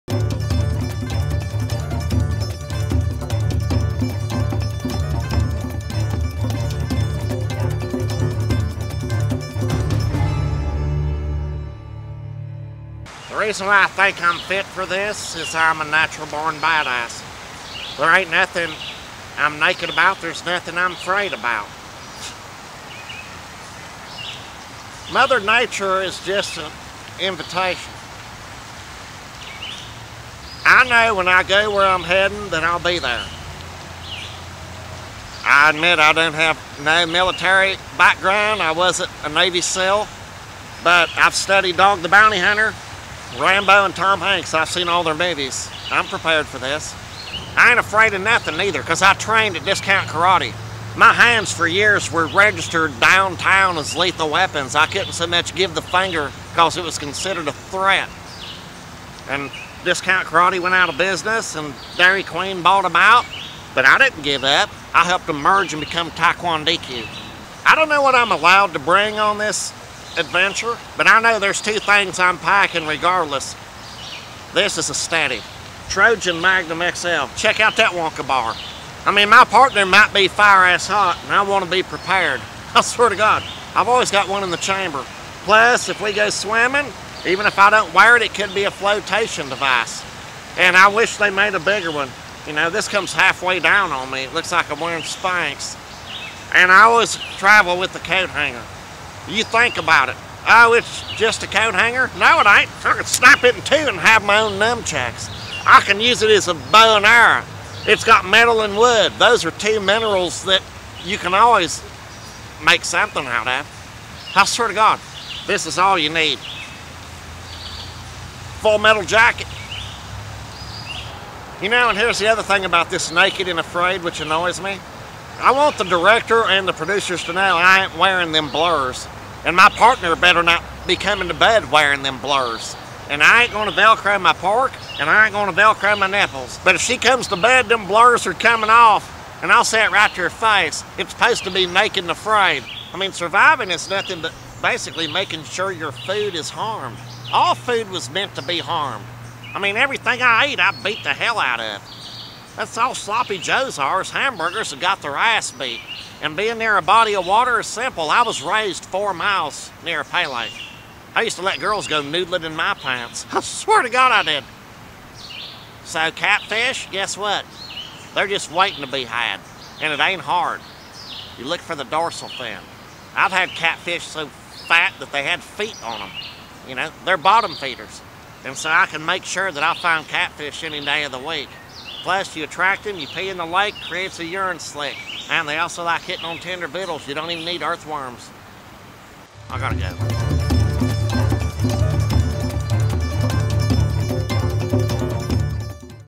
Donnie Baker Leaked Audition Tape for Reality Tv Show Naked and Afraid!
This video is about Donnie Baker auditioning for the reality show Naked and Afraid.  This audition tape has been leaked to the public against Donnie Baker's wishes. To watch Donnie open up about his inner most feelings click on this link to see his latest River Confessions:

Remember to follow me on Twitters and FaceBooks if you don't want to die alone: